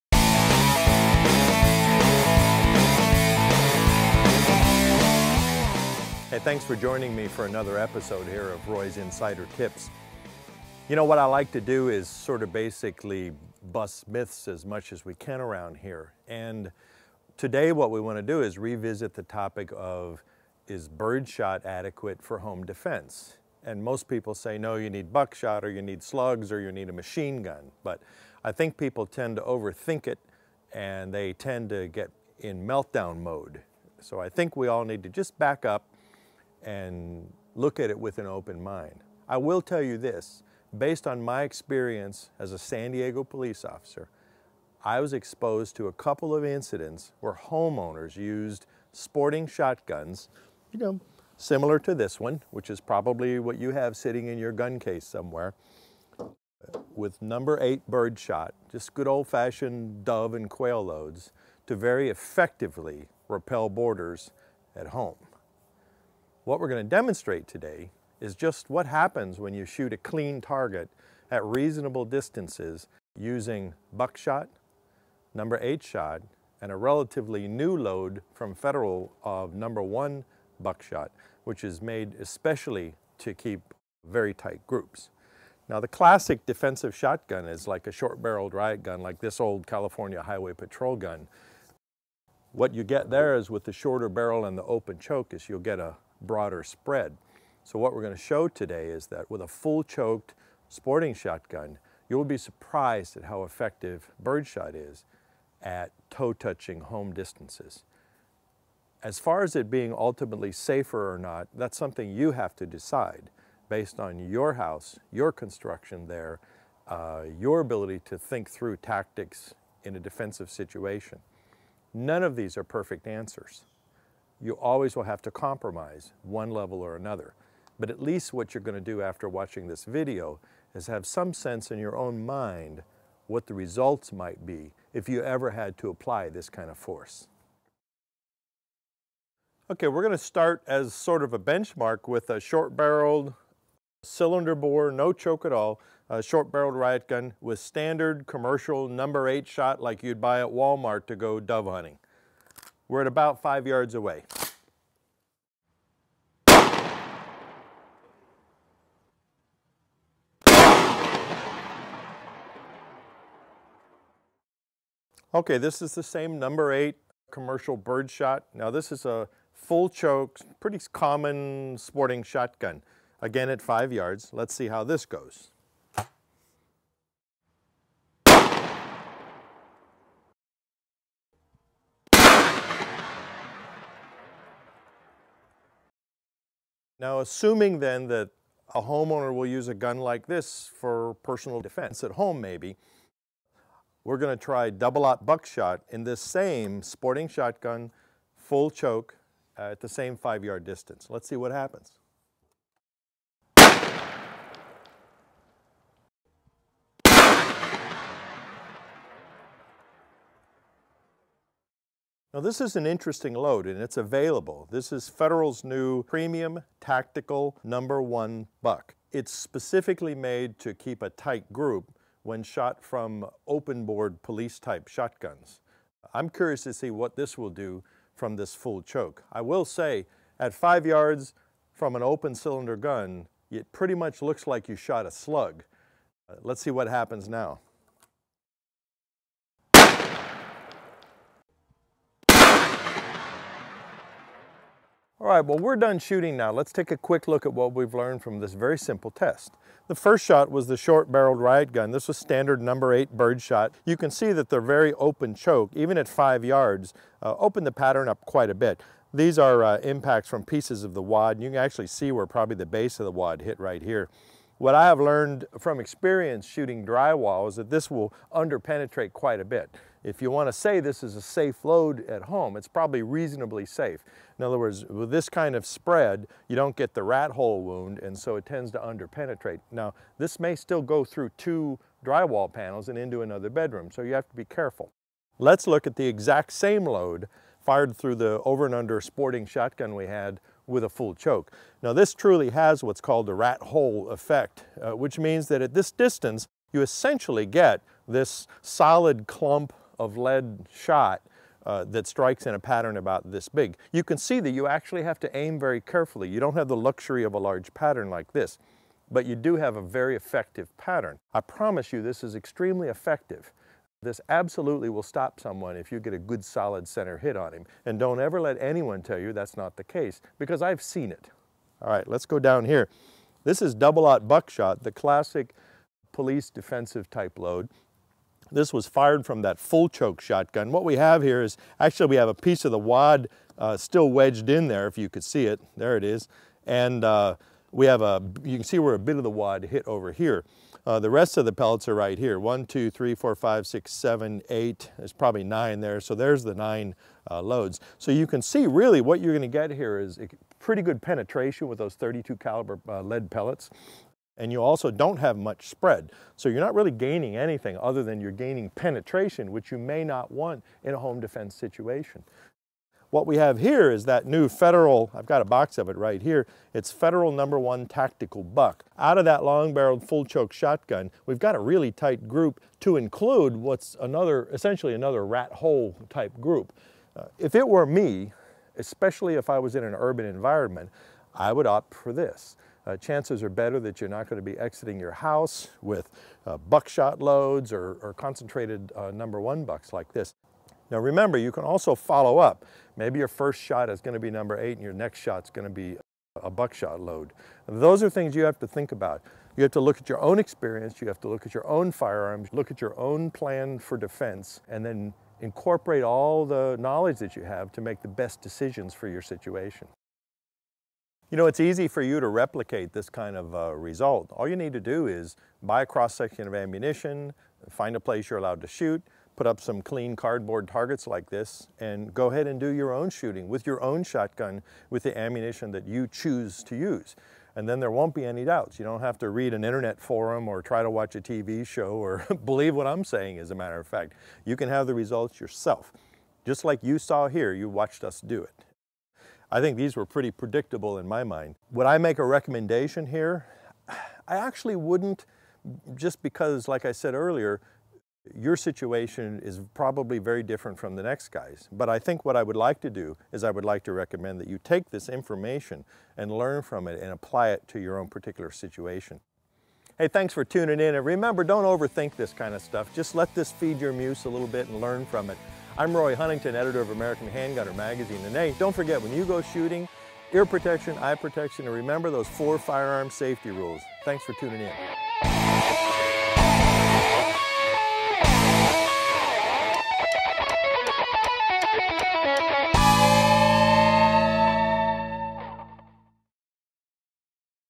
Sporting Shotguns For Home-Defense?
American Handgunner's Roy Huntington revisits the pointed topic of home-defense by looking at how different loads —  No. 8 Shot, 00 Buck and No. 1 Shot — hold up in a sporting shotgun. Which load would you prefer to defend your home?